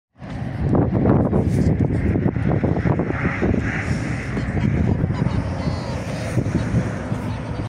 accidente camión A-33
Accidente de un camión en la A-33 a la altura de YEcla